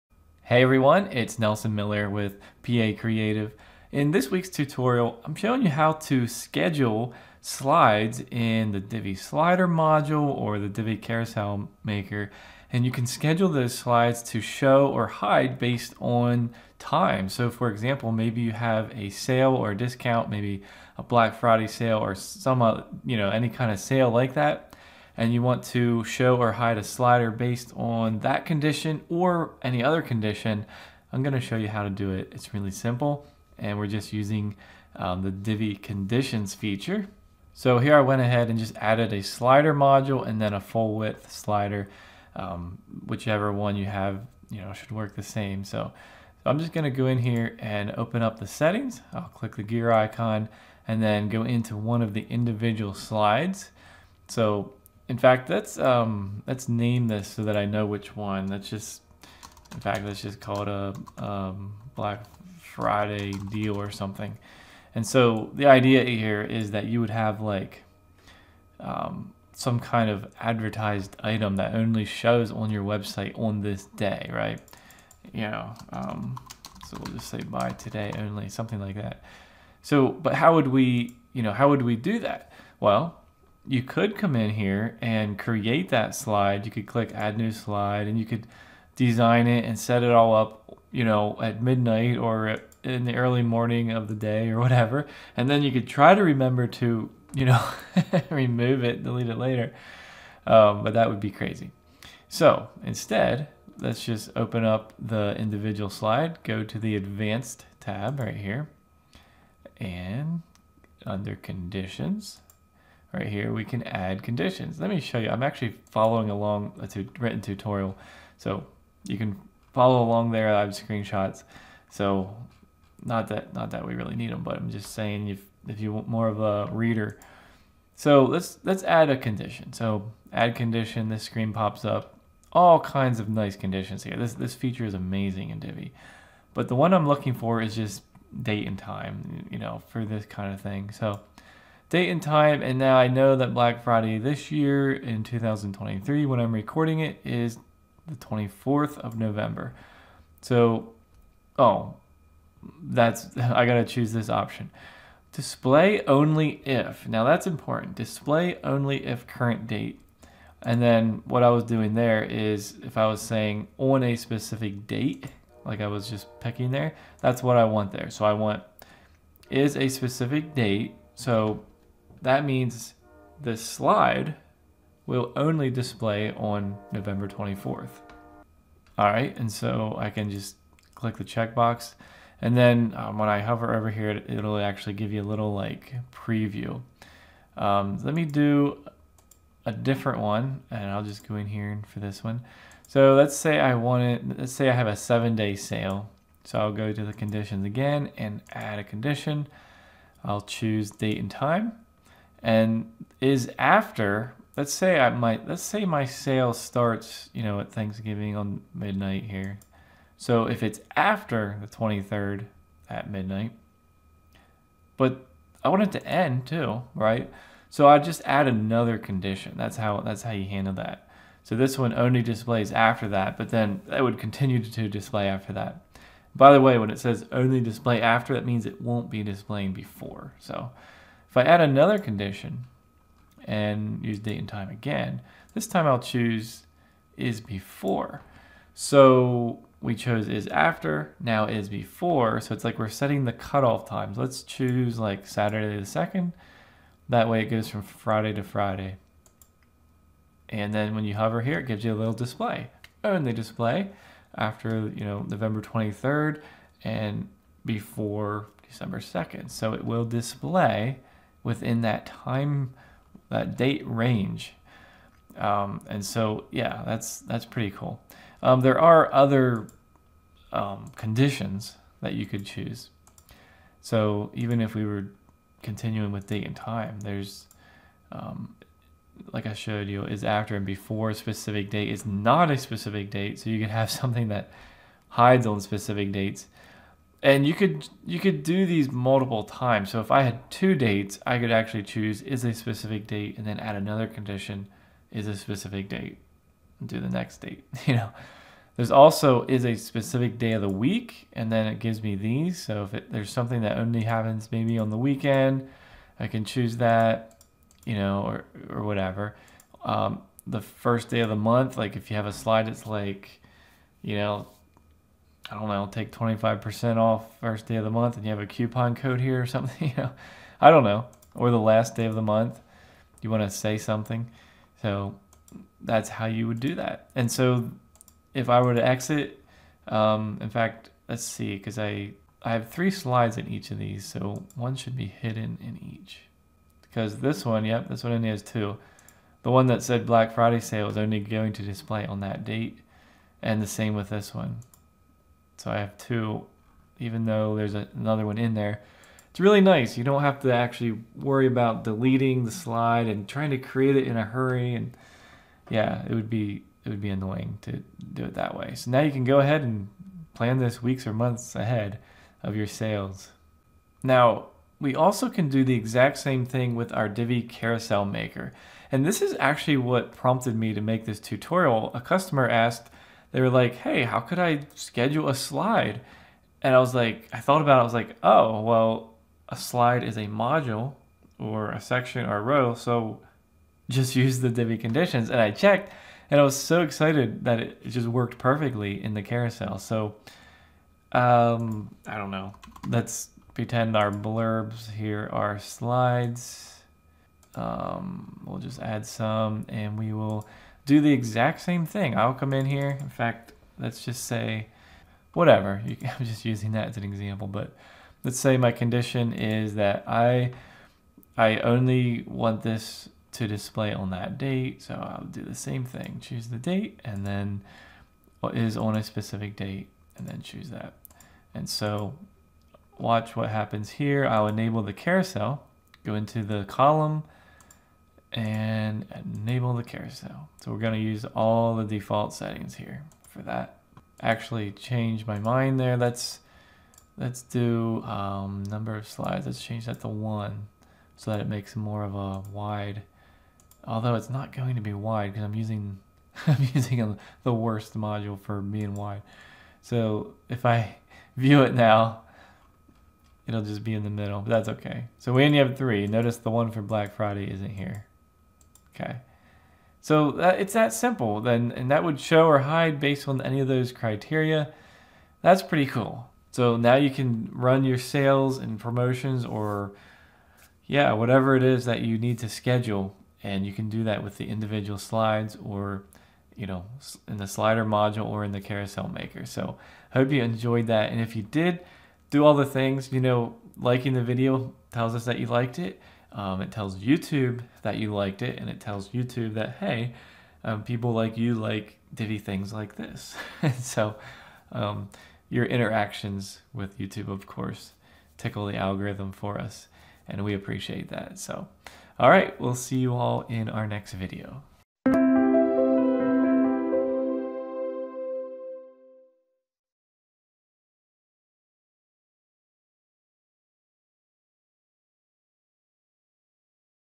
How To Schedule Slide Visibility In The Divi Slider Module (Or Divi Carousel Maker)
A customer recently asked me if they could schedule the slides in the Divi Carousel Maker plugin to show or hide based on date or other criteria. My first thought was to use check if this was possible using the Divi Conditions feature, and I was very happy to find that this worked perfectly! So in this tutorial, I will show you how to schedule slide visibility in the Divi Slider module and the Divi Carousel Maker plugin using the Divi Conditions feature.